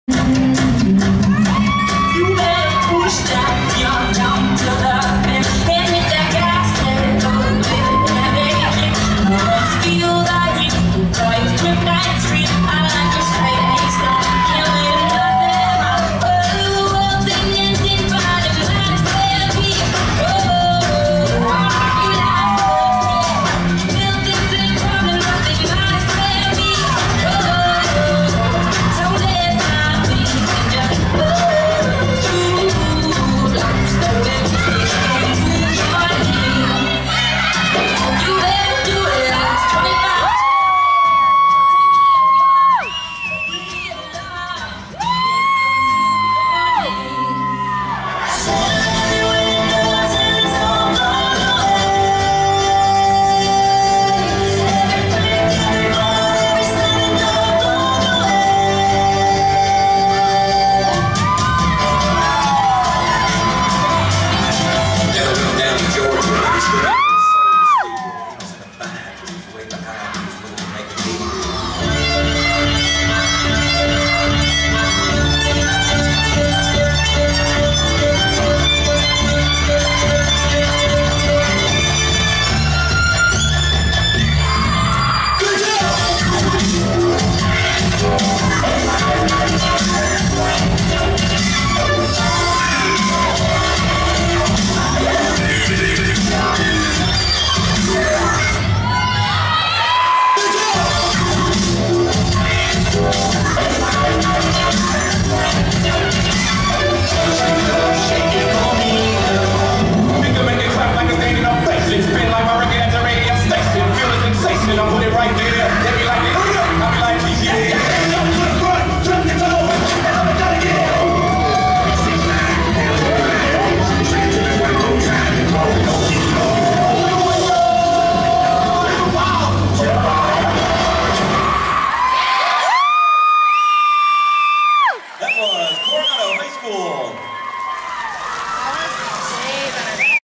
The Cougarettes Team Dance at the 2016 NDA National Championship (Finals)
The Coronado High School Cougarettes Dance Team